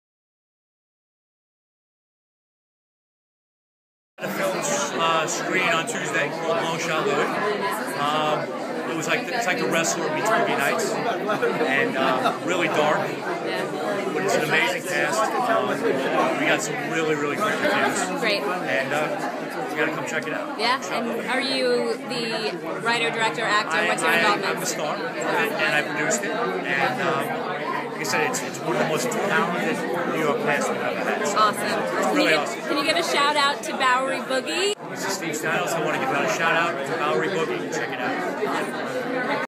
Soho Film Festival - Bowery Boogie Red Carpet Interview of Steve Stanulis
Steve is interviewed by Bowery Boogie on the 101 of Long Shot Louie.